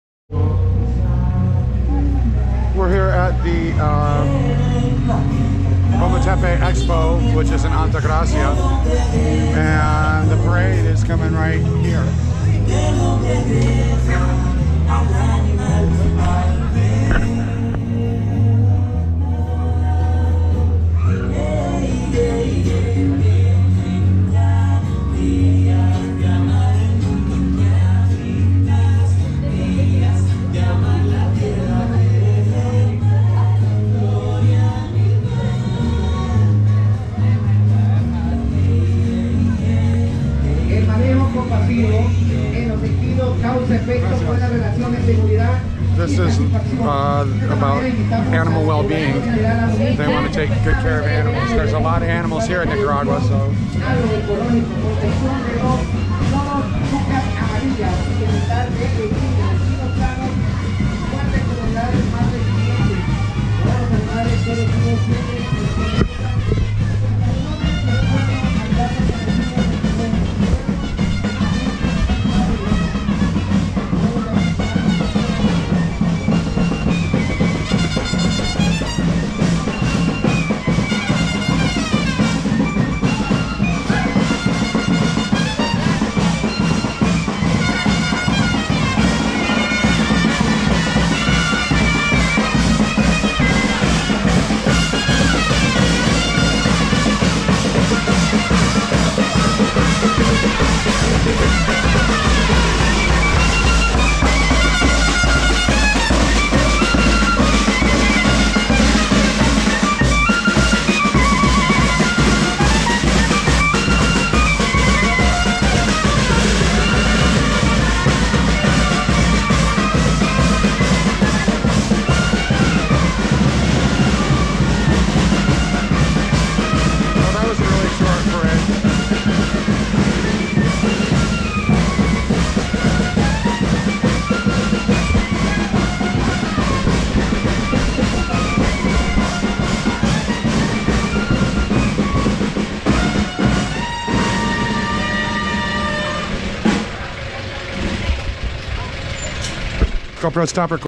Ometepe Expo 2022 Parade
This very short parade was in Altagracia for Expo Ometepe 2022.